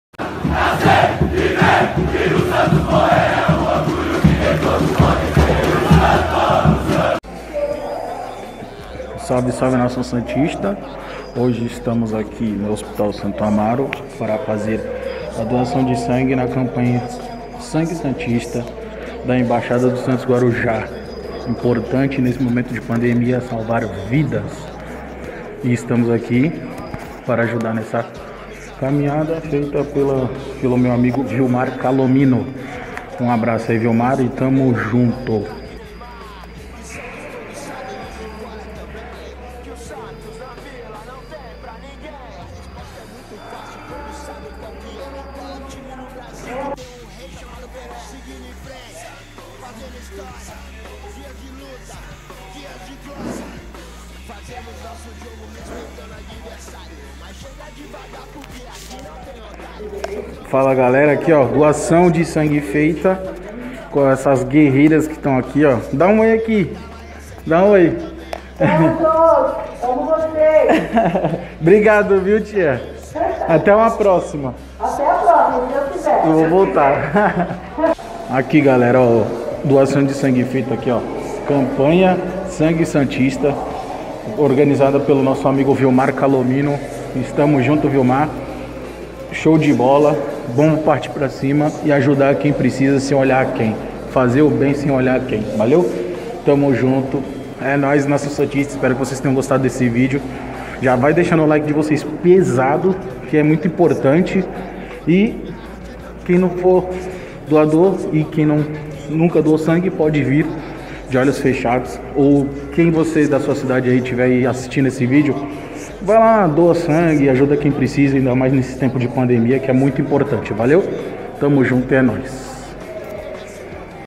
1° CAMPANHA SANGUE SANTISTA
Fala nação santista, o video de hoje é pra falarmos da 1° campanha sangue santista, sobre a importância de ajudar o próximo